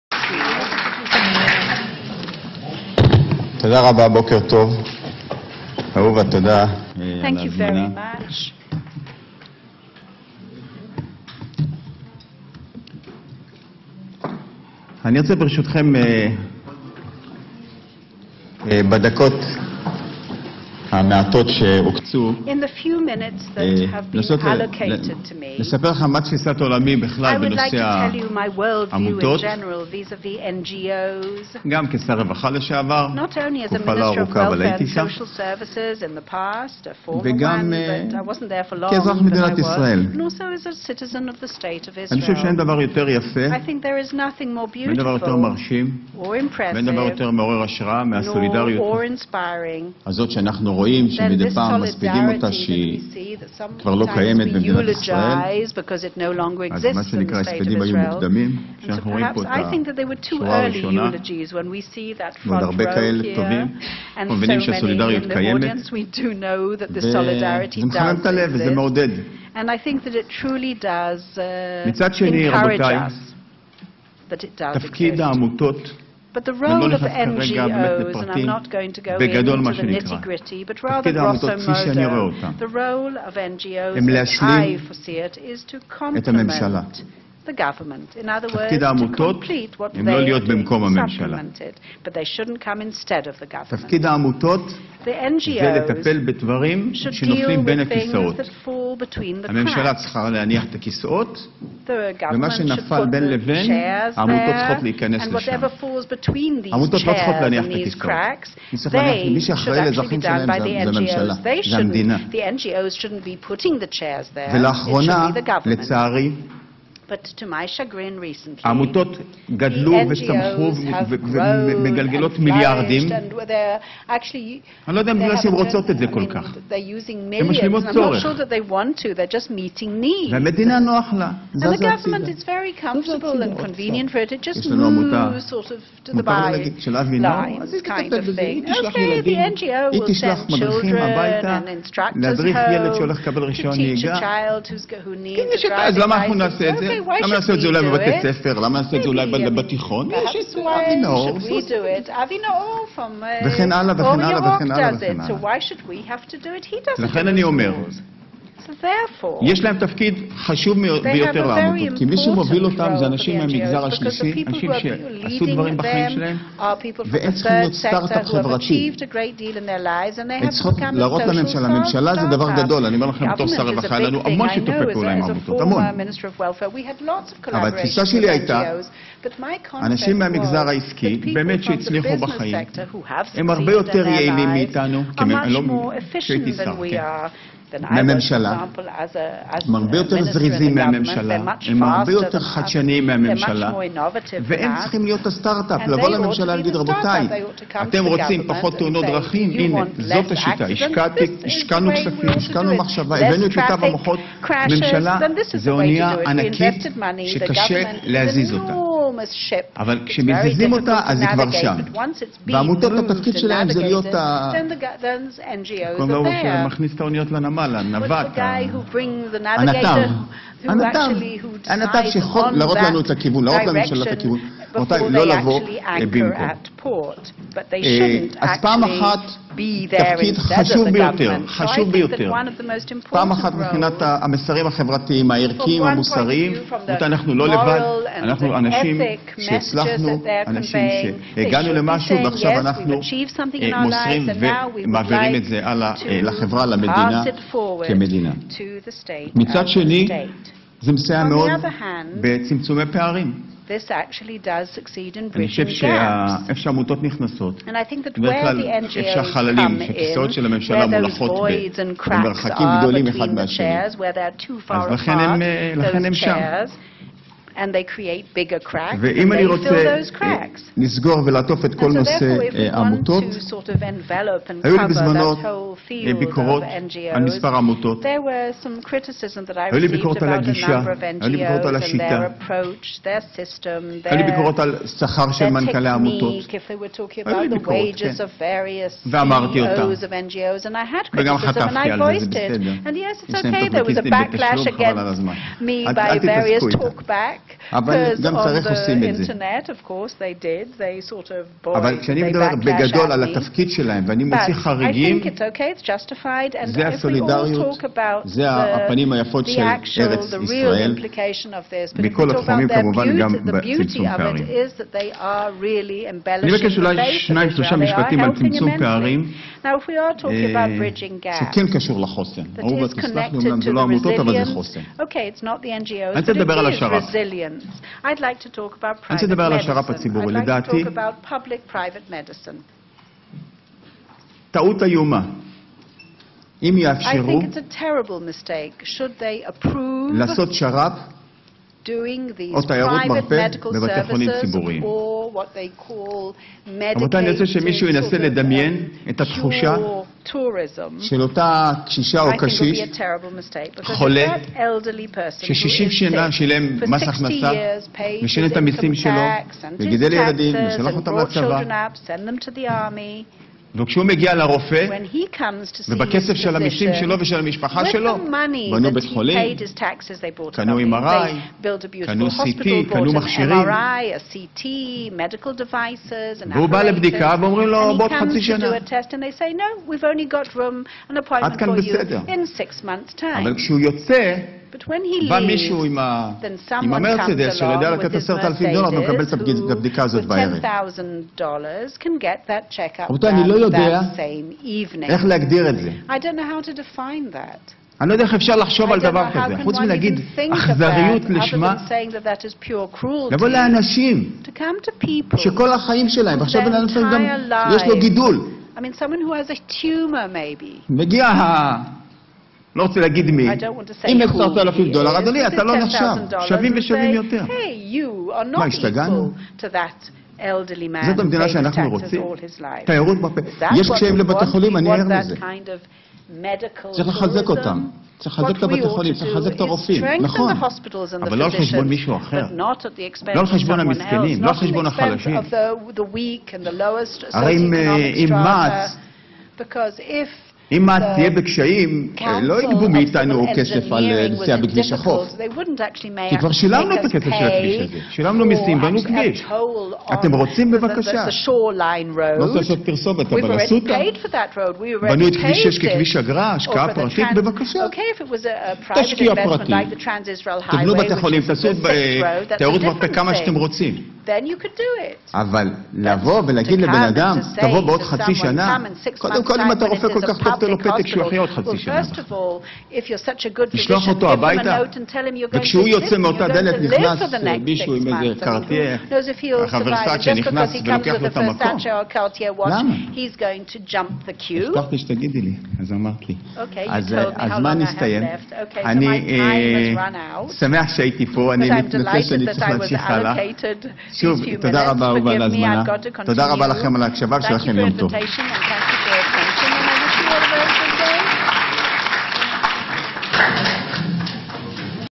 Mr. Moshe Kahlon, Chairman, Center for Reforms & Leadership, Netanya College, at Herzliya Conf 2014
Philanthropy in the 21st century and its implications for Israel: Emerging trends and new directions
Mr. Moshe Kahlon, Chairman, Center for Reforms and Leadership, Netanya Academic College, Former Minister of Communications and Former Minister of Welfare & Social Services